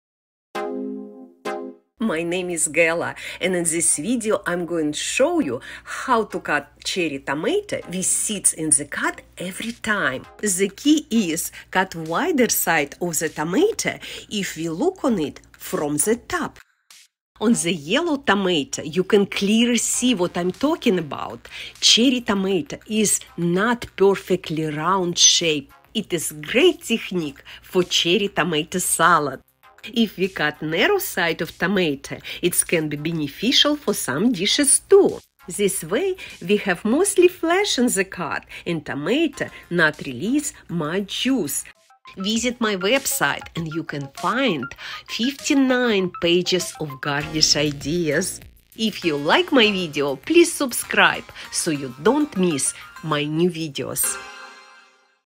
How To Cut Cherry Tomato With Seeds In The Cut Every Time For Salad And With Flash In the Cut
How to cut a cherry tomato with seeds in the cut every time is a great cherry tomato salad tip! If we cut in the opposite direction cherry tomato we have a flash in the cut every time for appetizers and the tomato doesn't release the juice. Tomatoes release more juice and look good on the salad as well! With this technique, we can cut cherry tomatoes with seeds in the cut and without seeds, depends what result we looking for. In my video, I show examples of how we practically use this technique. Cherry tomato is not a perfectly round shape and it is the key to how to cut cherry tomatoes for salad, appetizers, or just add to a plate as a garnish!
Update! I cut many small tomatoes and can say this technique works on almost all. However, I find one exception in yellow tomatoes If you are interested to see the photo and read more about the technique, visit my
0:10 Red cherry tomato cutting
0:18 Yellow cherry tomato I show the shape
0:31 How can be beneficial this technique cut a cherry tomato with more flash in the cut
0:36 The example of when better serve cherry tomato with more flash in the cut
Check more garnish ideas →

My cooking channel Galainthekitchen. My videos are about recipes, cooking techniques, and dishes decoration.
my hashtags are galasgarnishes